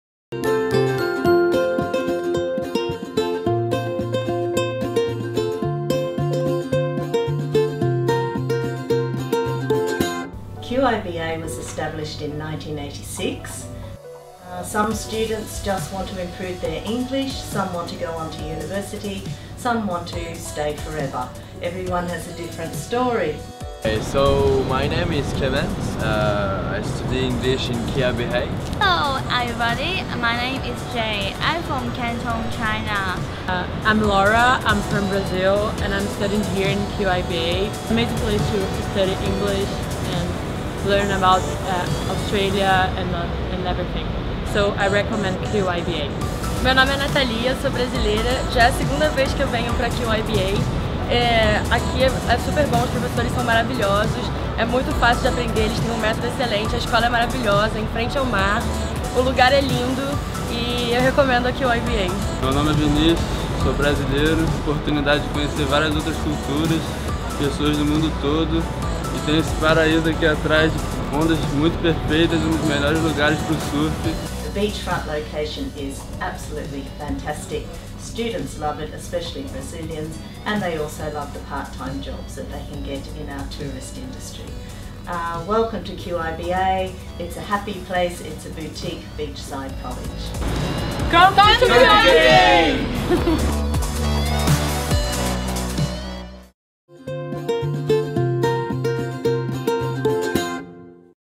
QIBA - Queensland International Business Academy
QIBA é uma parceira oficial da Southern Cross University e por mais de 20 anos e estabeleceu uma invejável reputação no mercado de educação, com cursos de inglês e Vocacionais.